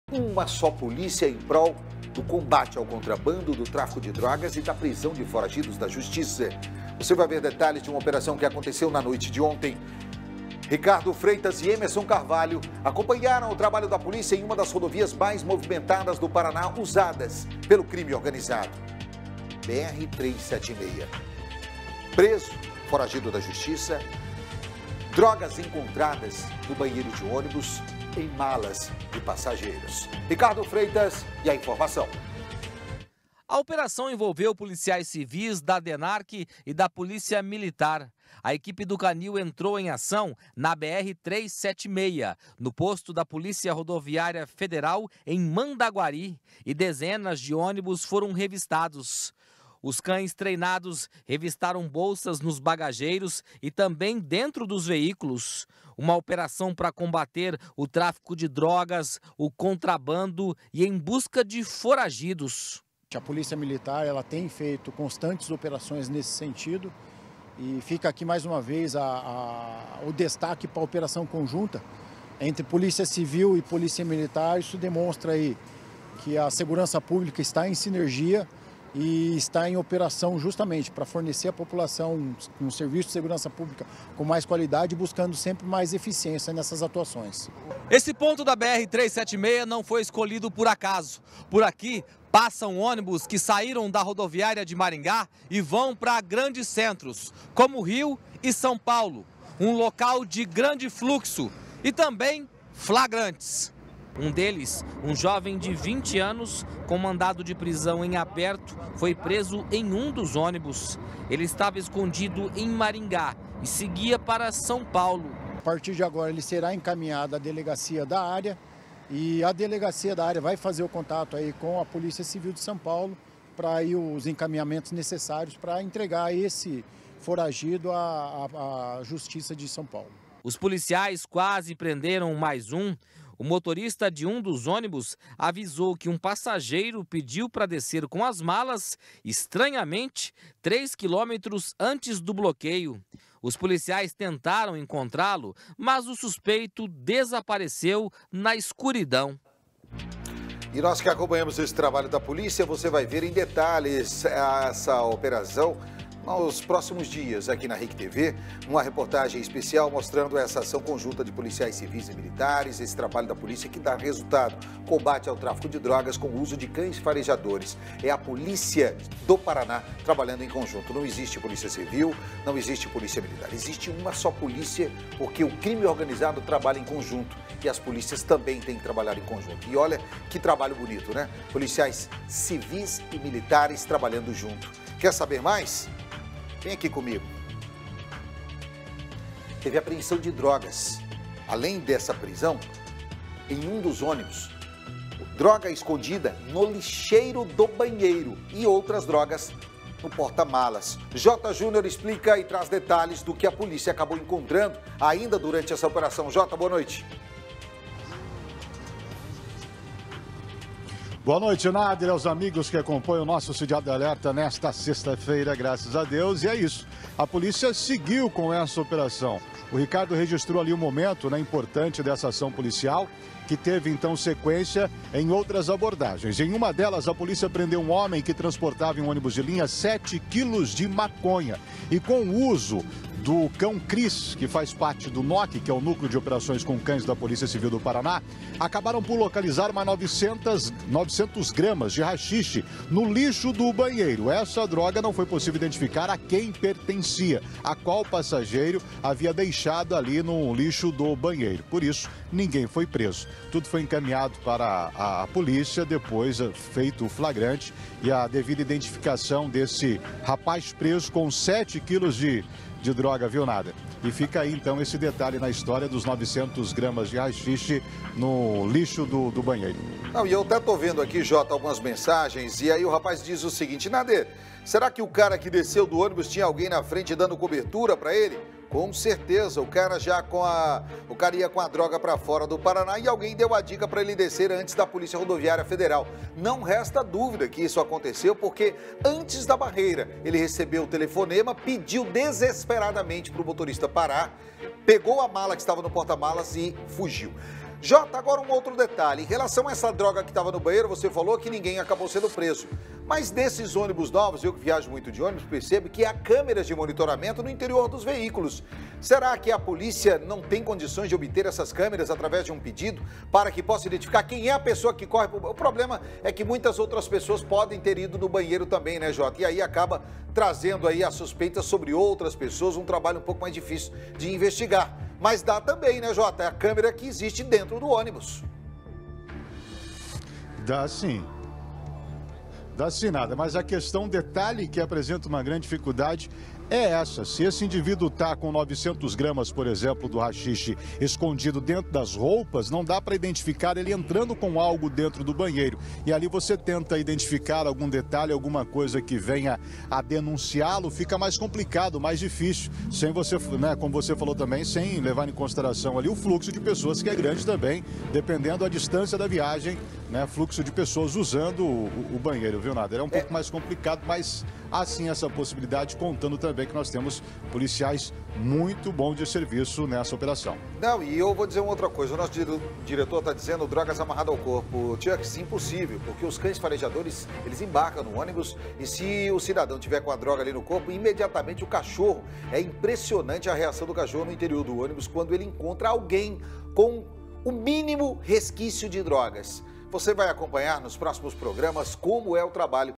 Operação com cães para combater o tráfico de drogas
Foram apreendidos 7 quilos de maconha com homem em ônibus de linha, próximo a Mandaguari. Confira todos os detalhes na reportagem!